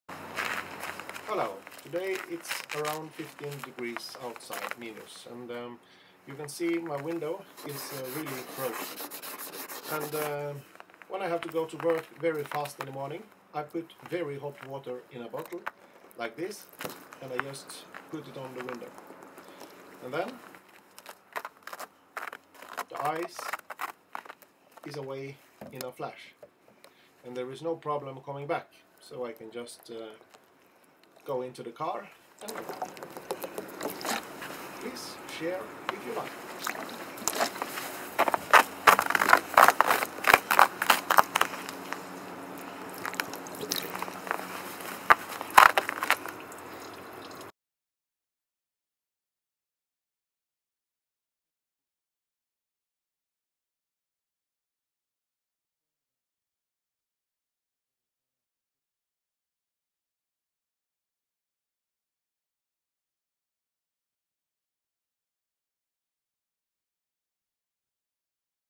How To get rid of Ice on Windshield when your front window is frozen
How to remove ICE on the front windows in 5 seconds when it´s cold outside and your front window is frozen. I have done this for at least 7 years every morning when it´s ice on my front window and have never  had any problems with it. Fill a bottle, around 1 or 1,5 liter with hot water from your kitchen.